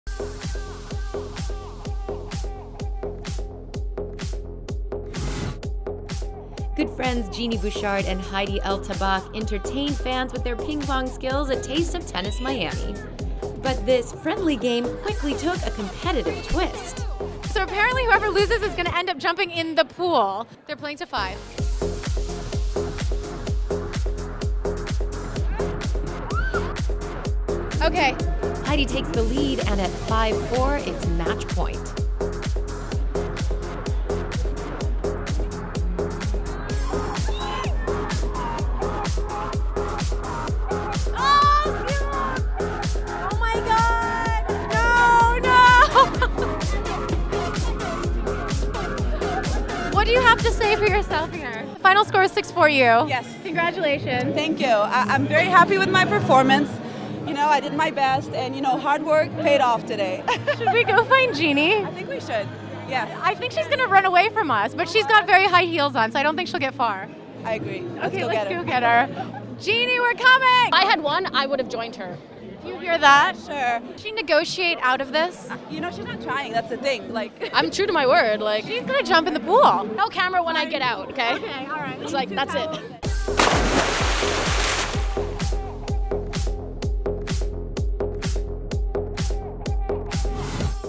Genie Bouchard and Heidi El Tabakh Show Off Their Ping Pong Skills
You can take the tennis player away from the court, but you can’t take the competitive spirit out of the athlete! Canadians Genie Bouchard and Heidi El Tabakh competed in a fun game of Ping Pong during the Taste of Tennis event in Miami. What began as a playful bout of table tennis turned into a high stakes battle that would end in the swimming pool.

Heidi was in the lead and the pressure was on Genie to stay in the game, but with a ping pong ball in the net, the match was over at 6-4. With visions of smashing the paddle into pieces, Bouchard had to accept defeat!

Host Mayleen Ramey joined champion Heidi El Tabakh to get her thoughts on the match. “I’m very happy with my performance. You know I did my best and hard work payed off today!” The direction turned to Bouchard and questions arose on whether or not the tennis star was going to take the final plunge. “I’m true to my word! Just no camera when I get out, ok?”

Watch the night unfold for Genie and Heidi at a safe distance away from the splash zone!

New videos every TennisTuesday!